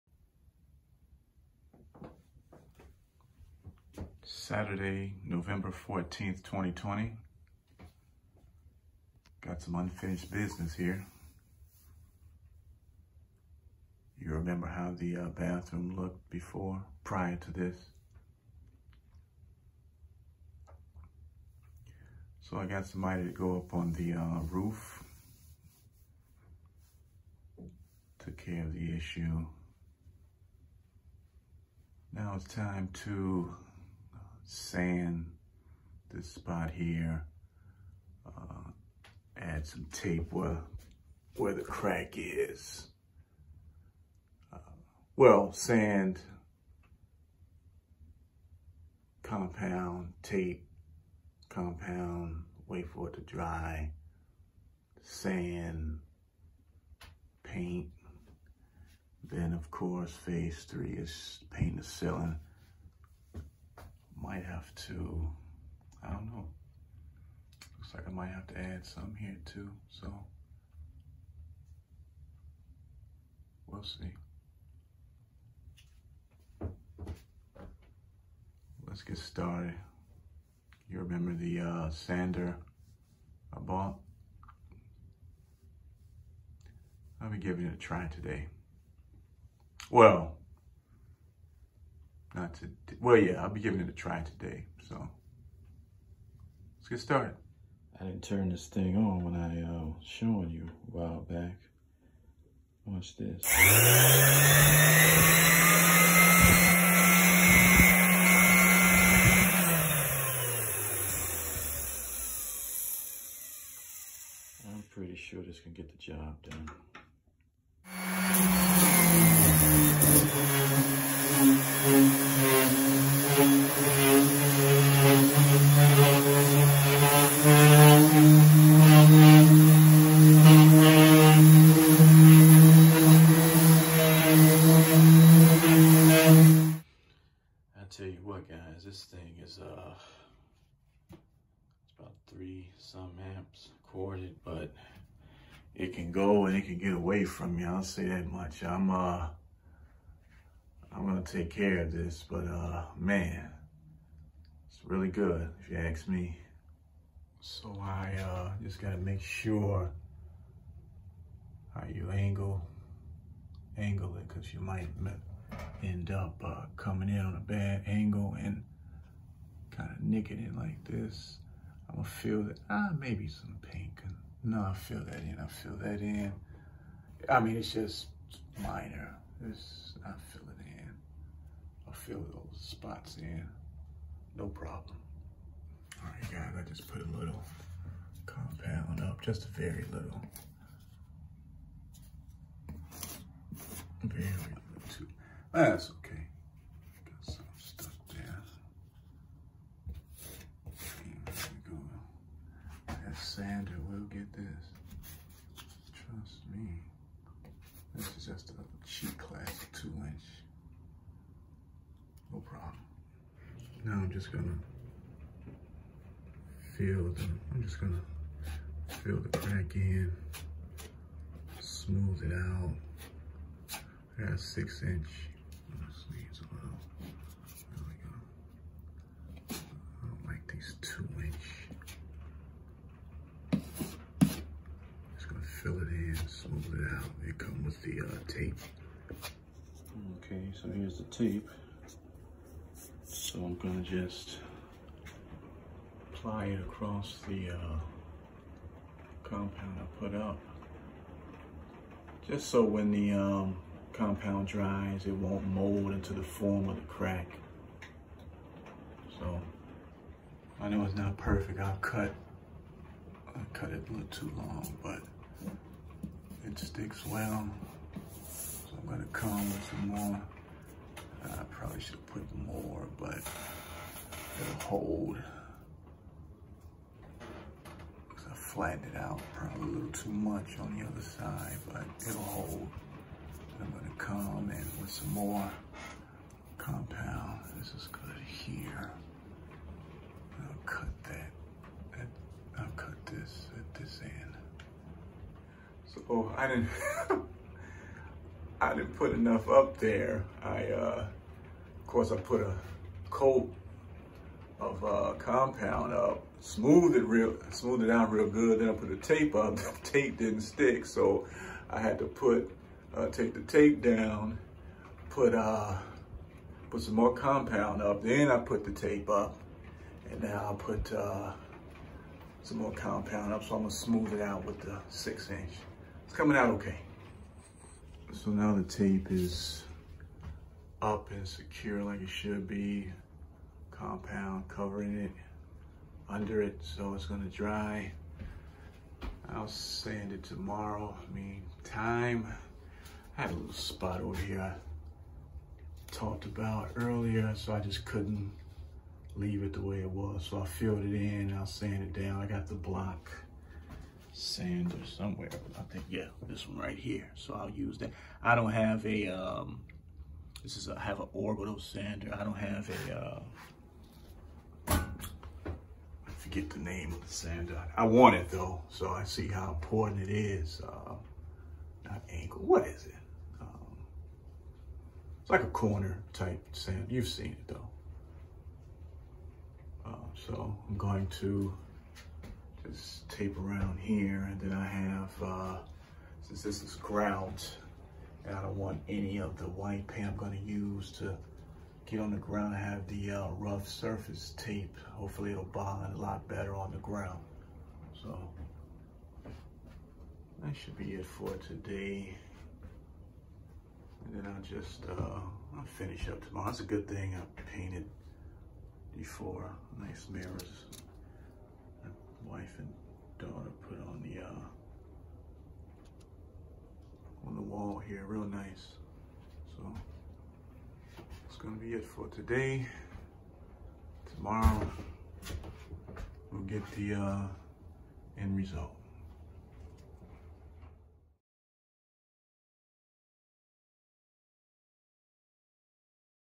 Drywall Repair feat. Tool Shop Orbital Sander (Pt. 1)
With the use of an orbital sander, Whitley Green repairs a crack in the ceiling!